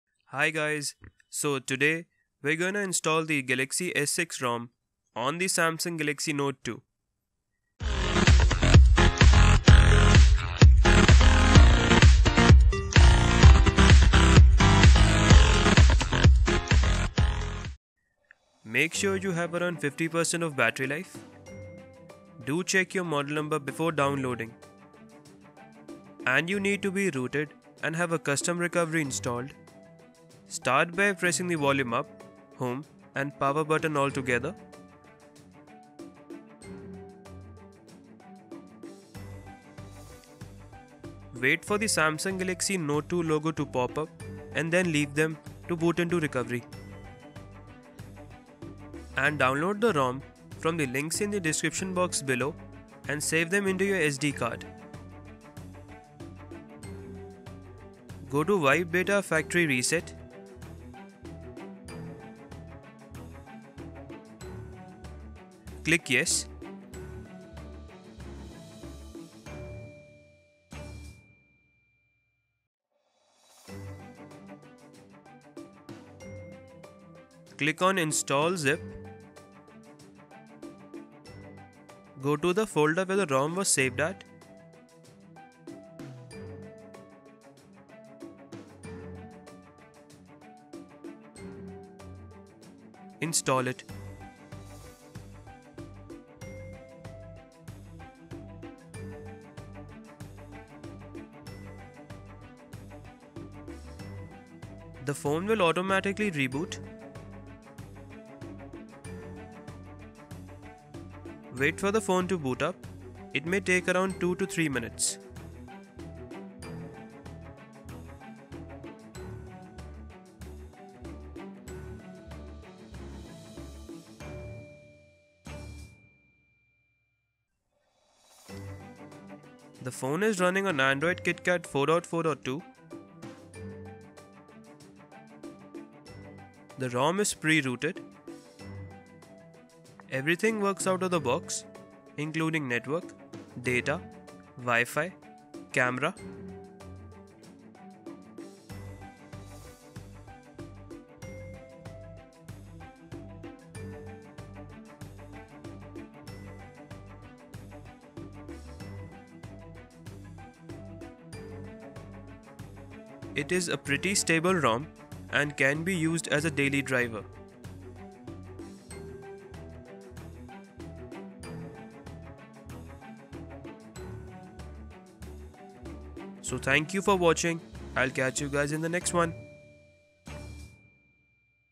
Install Galaxy S6 Rom On Samsung Galaxy Note-2!
Here's How to install Galaxy S6 Rom on the Samsung Galaxy Note-2 by Tech Tube!.

Save this to the SD card & carefully follow instructions to install Galaxy S6 Rom on your Samsung Galaxy note-2!